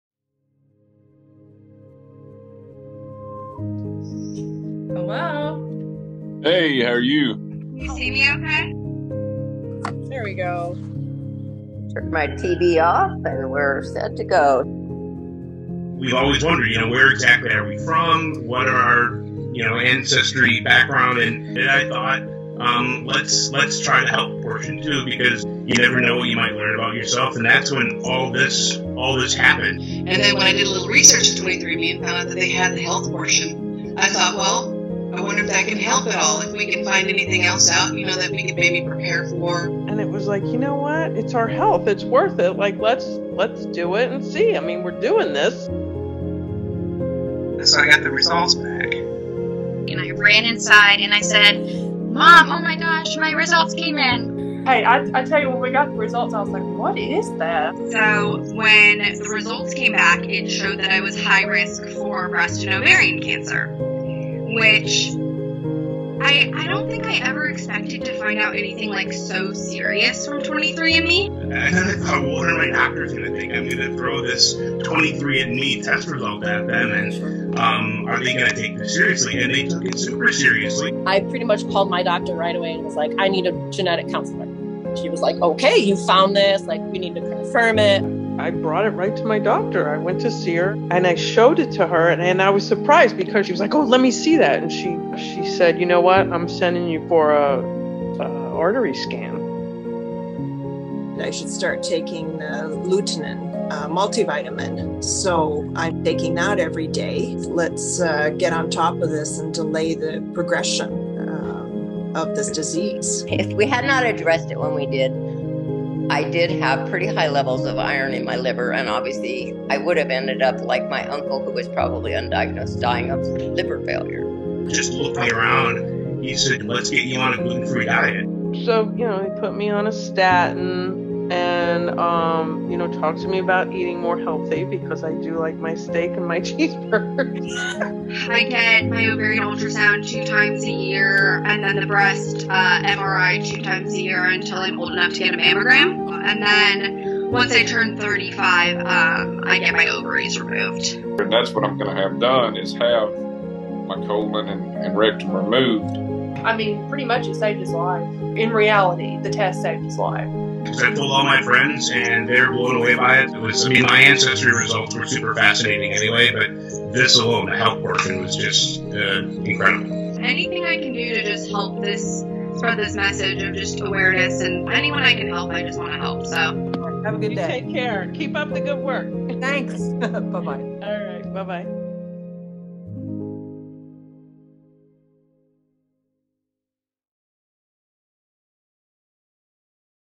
2023: Our journey together
As we move in to 2024, we wanted to share some of our favorite customer stories, in their own words.